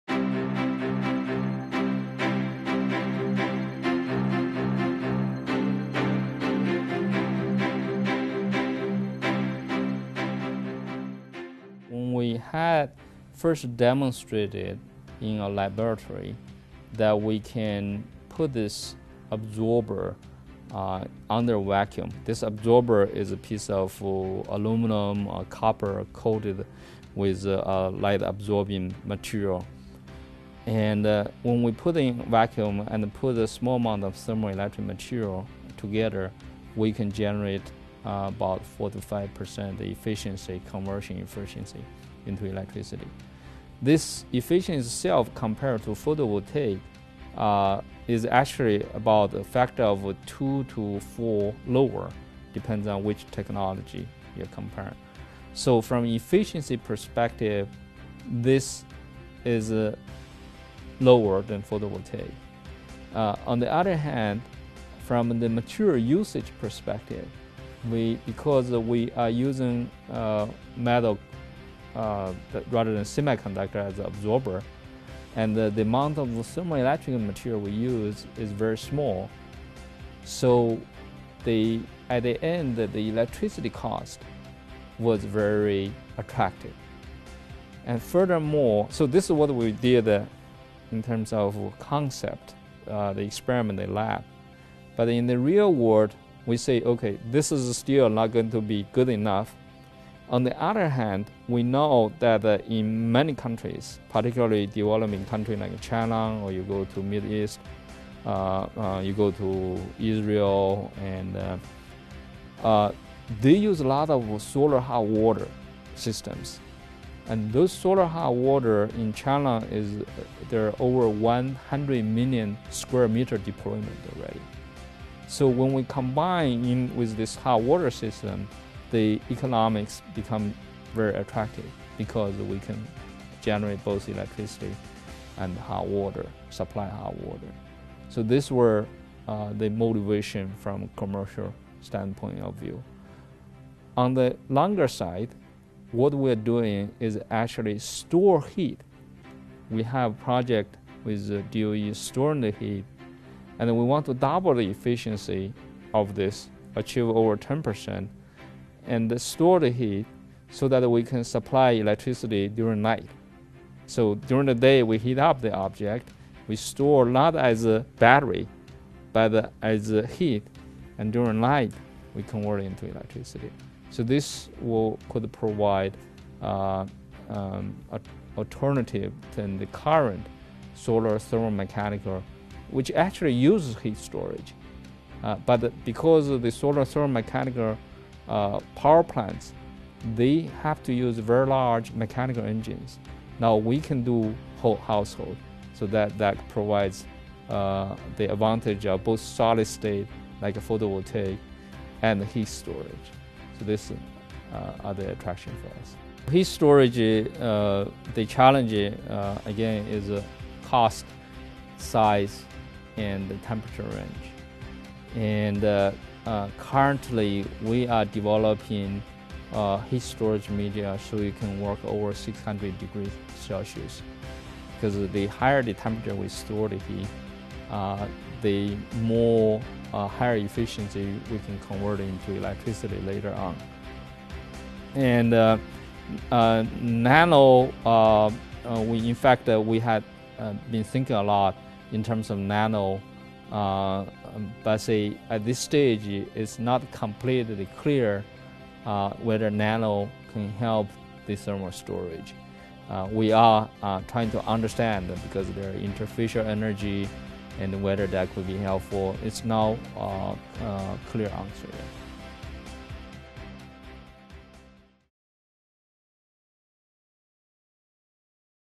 5-Gang Chen: Advantages of Thermo Electric Generators
MIT's Solid State Solar Thermal Energy Conversion Center Director Gang Chen discusses advantages of thermo electric generators.